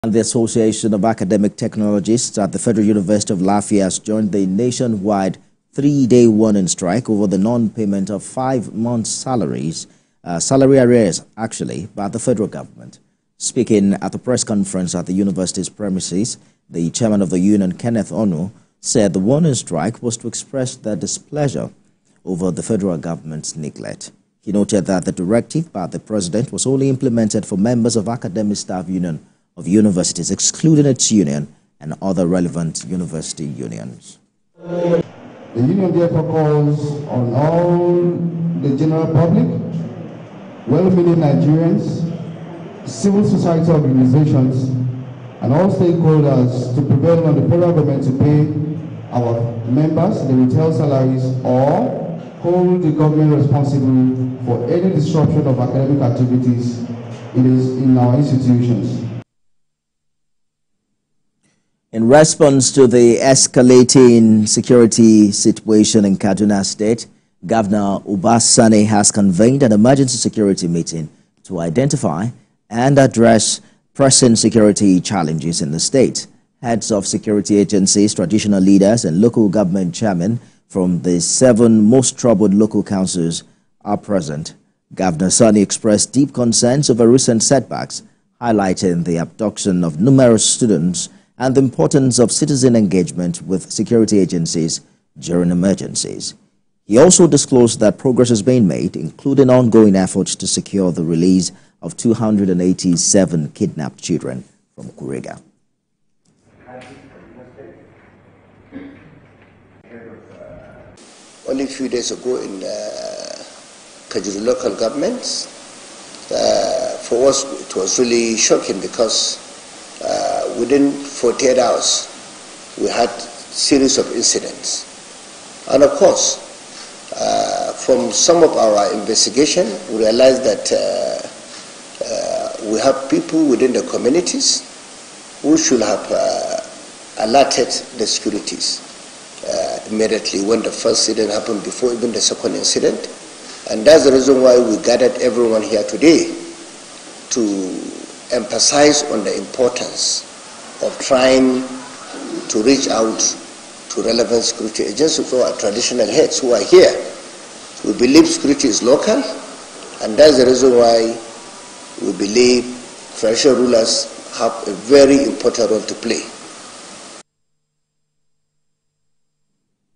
National Association Of Academic Technologist In Lafia Join Strike
The Association of Academic Technologists at the Federal University of Lafia has joined the Nation-wide three-days warning strike over the non-payment five months salary arrears by the federal government.
Speaking at a press conference at the university's premises, the Chairman of union, Kenneth Onuh, said the warning strike was to express their displeasure over the federal government's neglect.
He noted that the directive of the President was only implemented for members of Academic Staff Union of Universities, excluding its union and other relevant university unions.